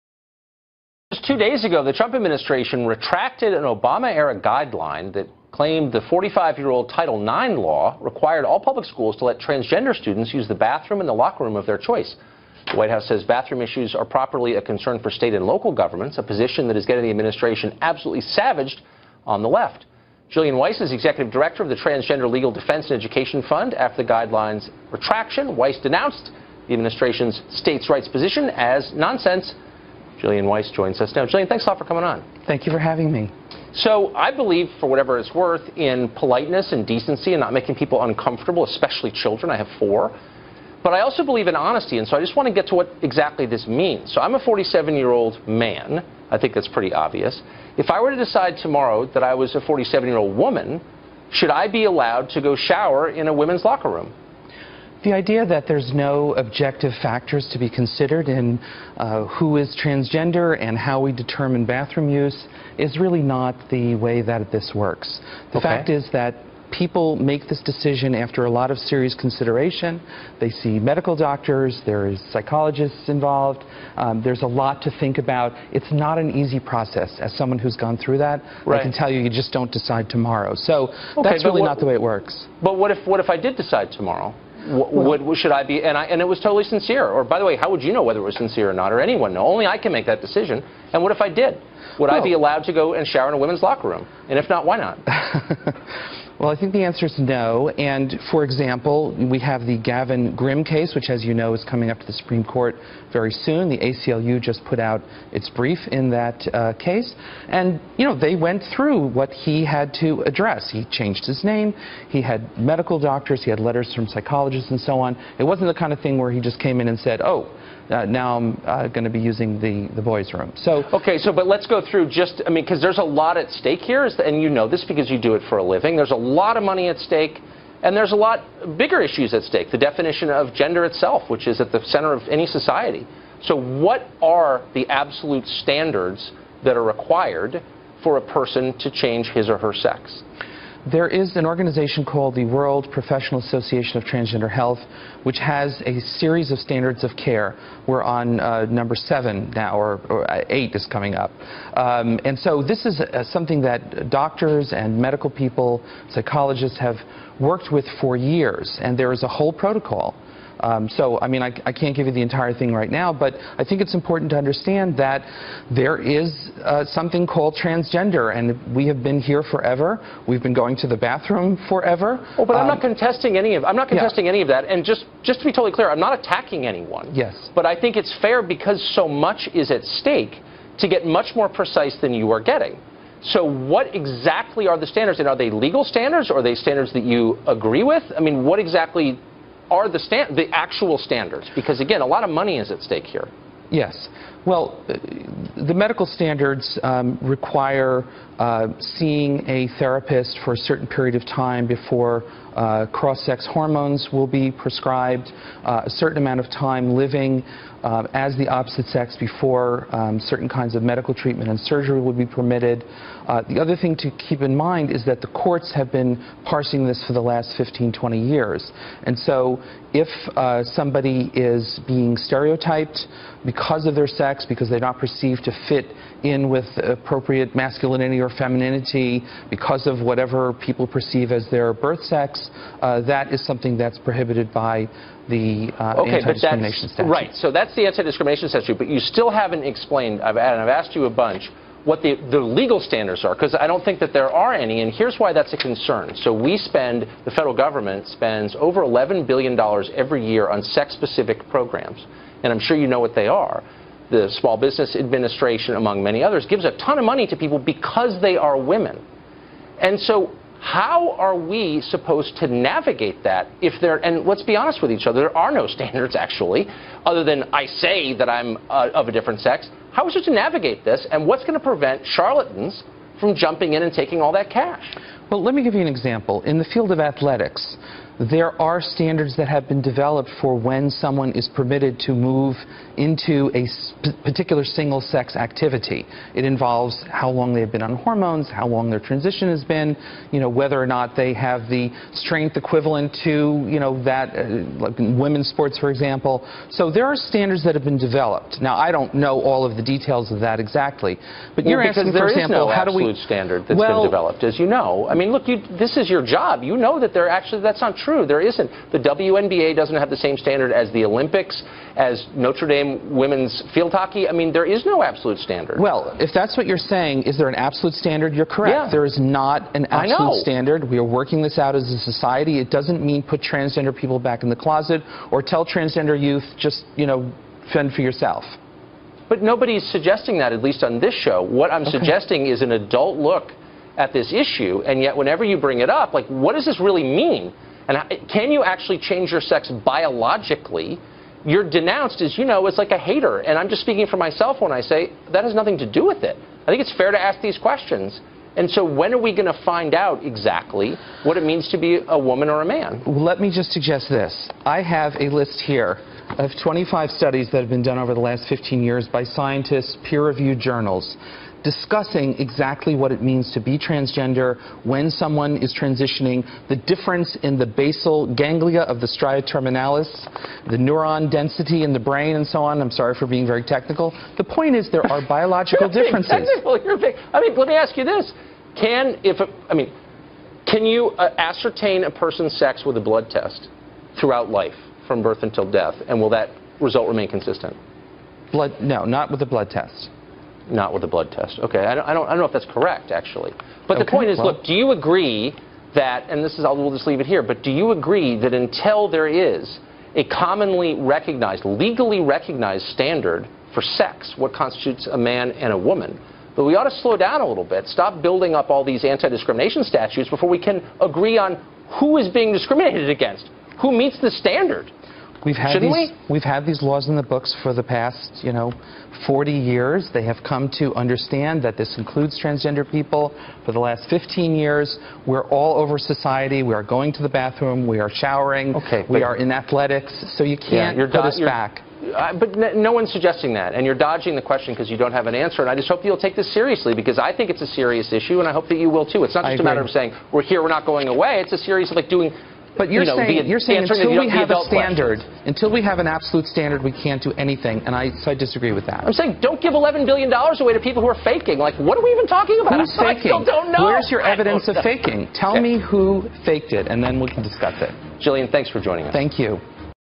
ANOTHER INTERESTING TUCKER CARLSON INTERVIEW
TUCKER INTERVIEWS A CONFUSED TRANSGENDER AND ASKS QUESTIONS ABOUT STANDARDS FOR LEGISLATION ON THE SUBJECT.. YOU CANNOT PASS LAW ON SUBJECTIVE THOUGHT, ONLY ON SCIENCE BACKED BIOLOGICAL SEX... WE DO NOT BASE LAW ON GENDER, WE ONLY BASE LAW ON PHYSICAL SEX.